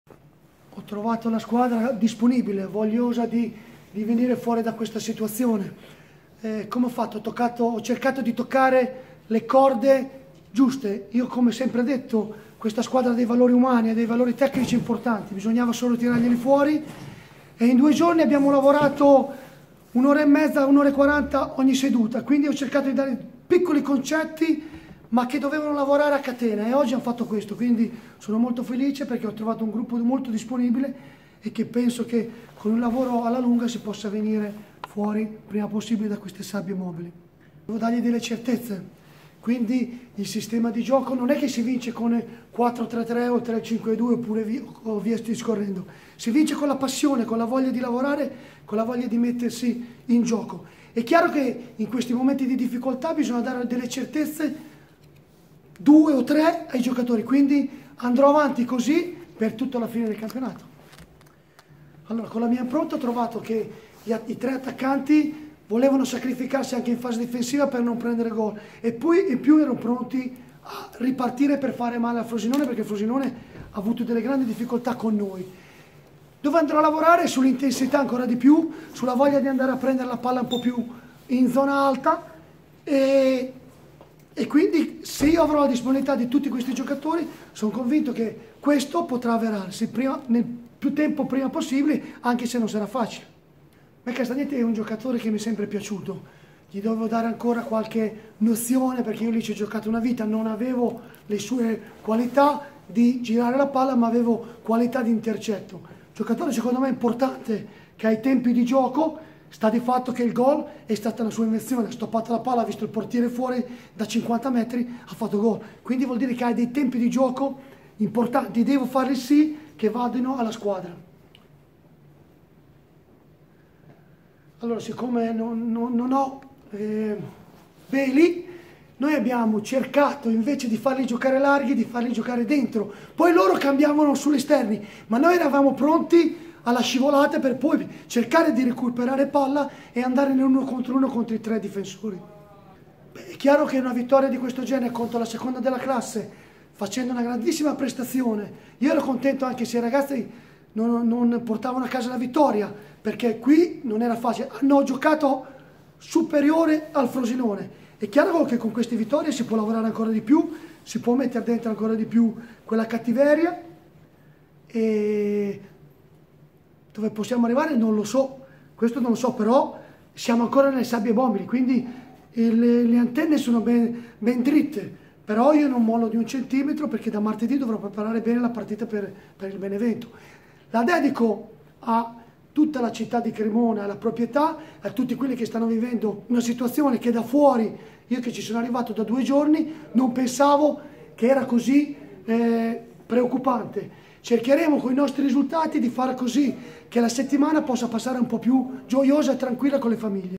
SALA STAMPA Bisoli: "Applicati pochi, fondamentali concetti"
Nelle immagini Pierpaolo Bisoli al termine di Frosinone-Cremonese, 28a giornata del campionato SerieBKT

Benvenuto nel canale YouTube ufficiale dell'U.S. Cremonese.